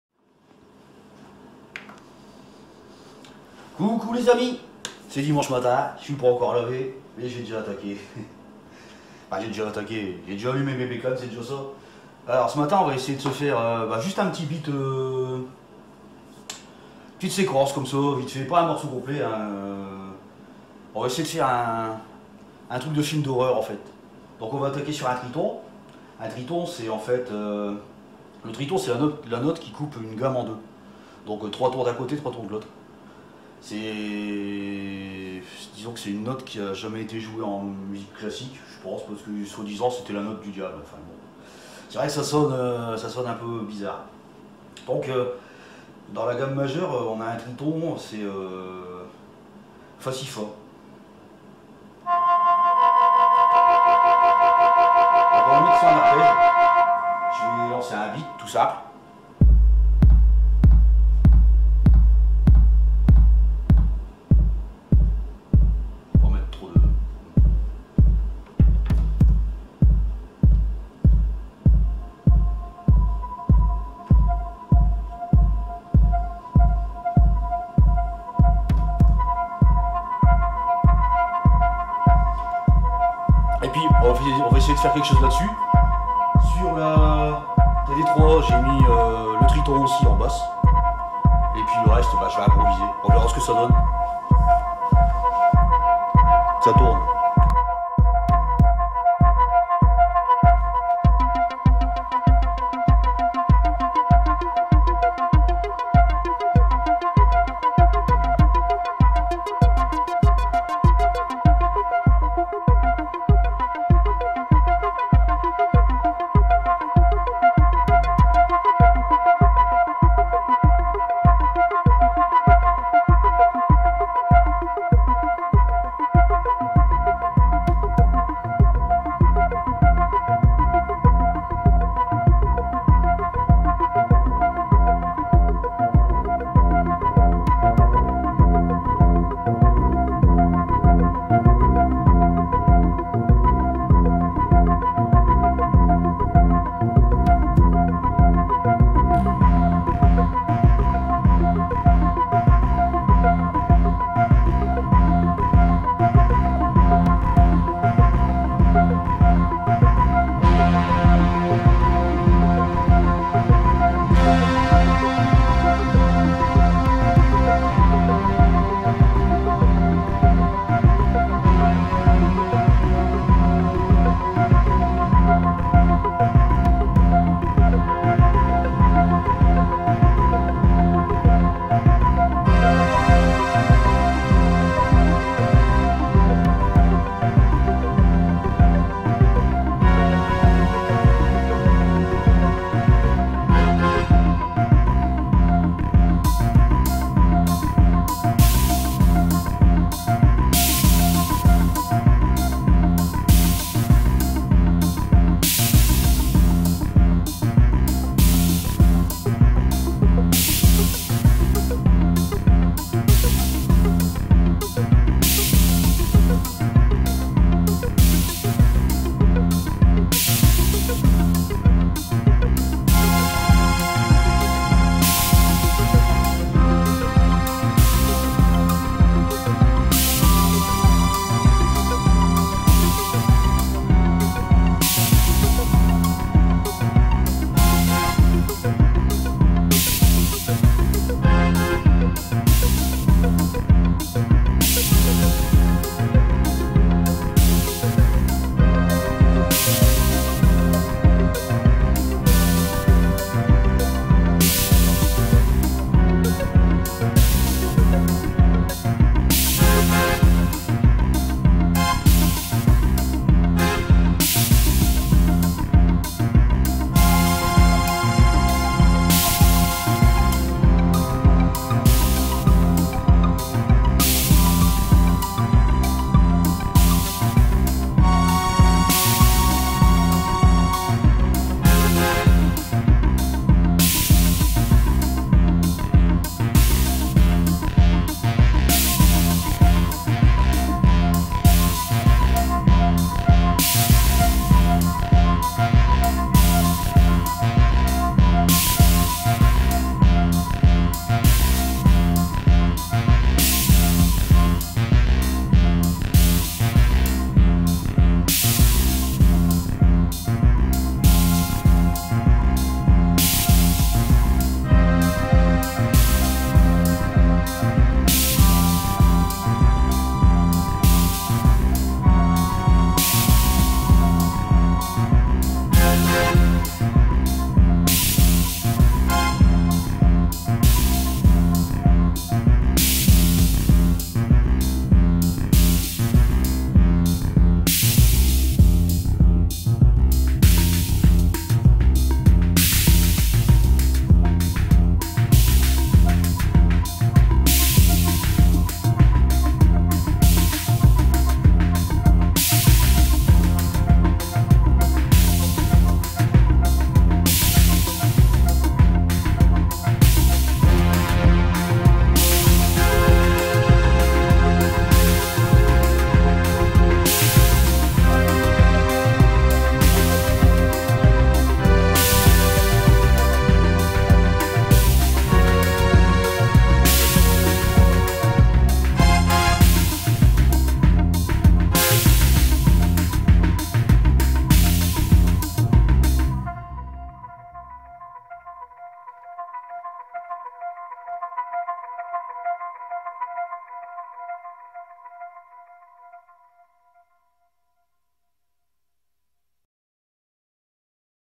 Jam improvisation d'une sequence film d' horreur Triton Microfreak drumbrute impact TD_3 Mininova
soutenez la chaine sur Tipeee et accedez à des exclusivités.